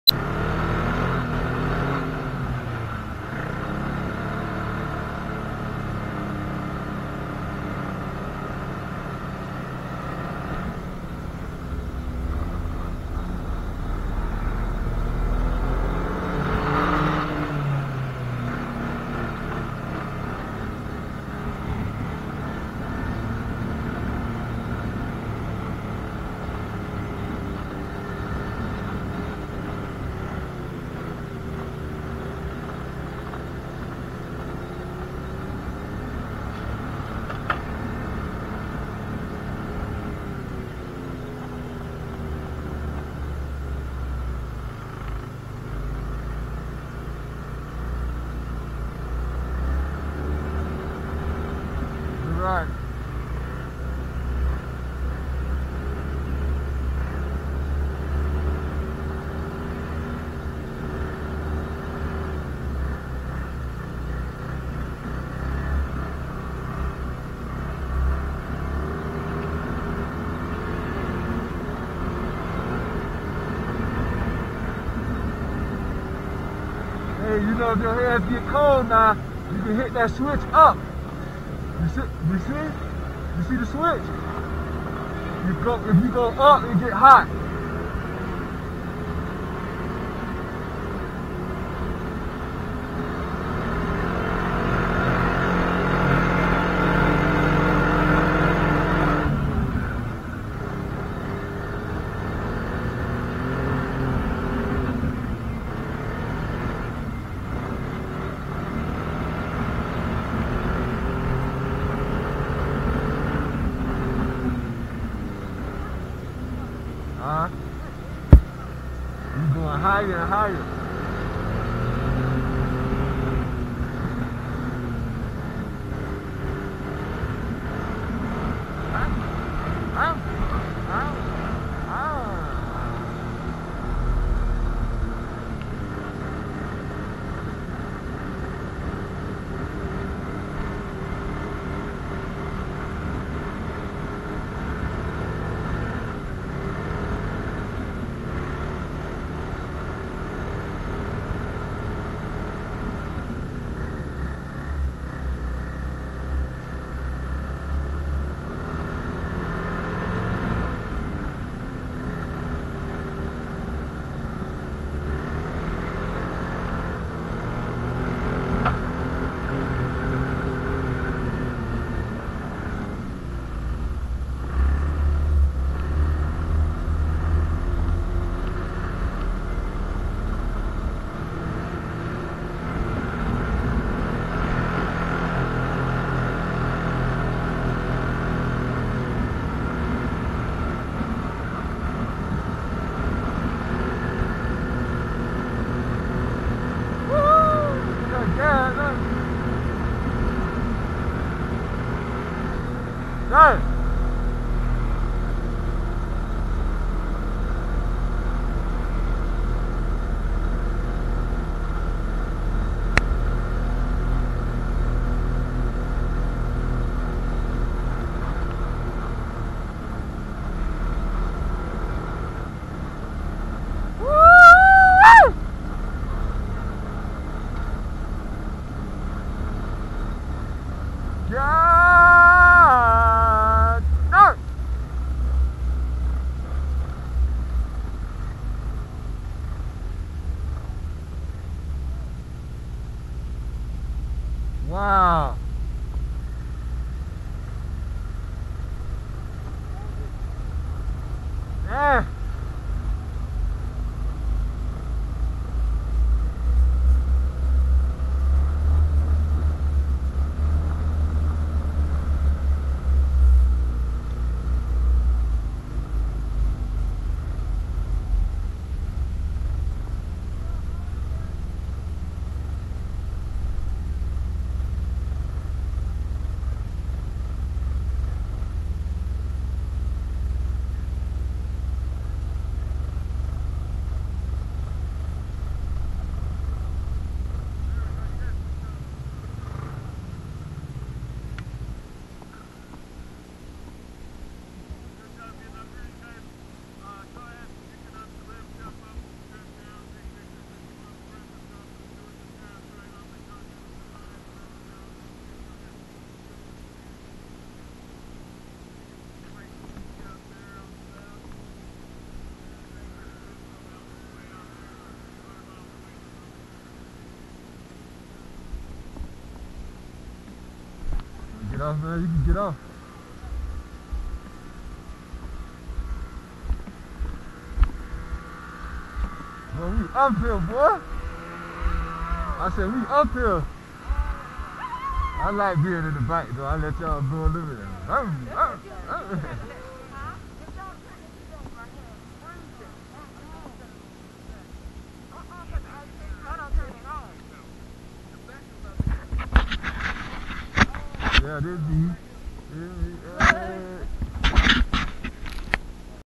The Johnsons Christmas Vacation in Breckenridge Colorado 2016
The Johnsons are on the snowmobiles having a ball headed up to the top of the mountain